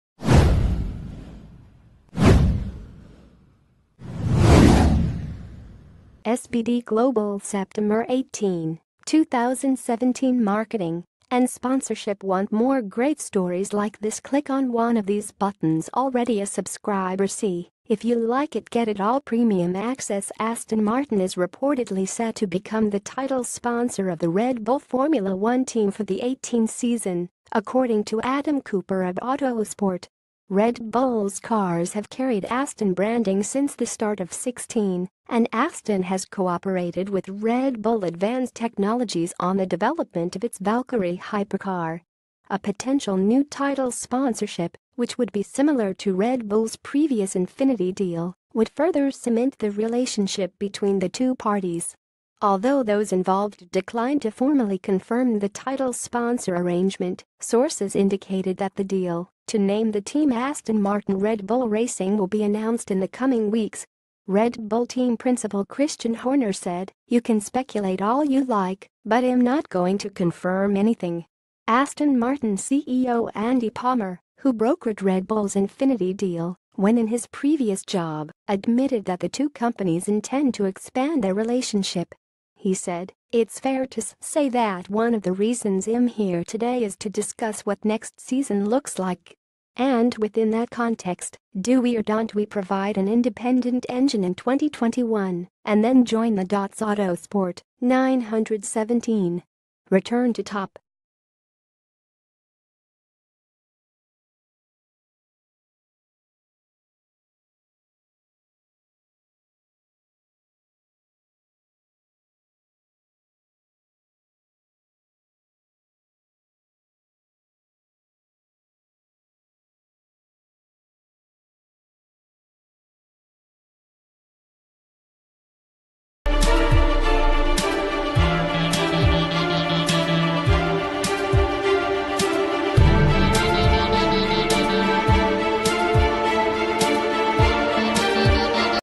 Breaking News | Aston martin reportedly will take over as title sponsor of red bull f1 in '18
Aston martin reportedly will take over as title sponsor of red bull f1 in '18

SBD GlobalSeptember 18, 2017Marketing and Sponsorship WANT MORE GREAT STORIES LIKE THIS? CLICK ON ON...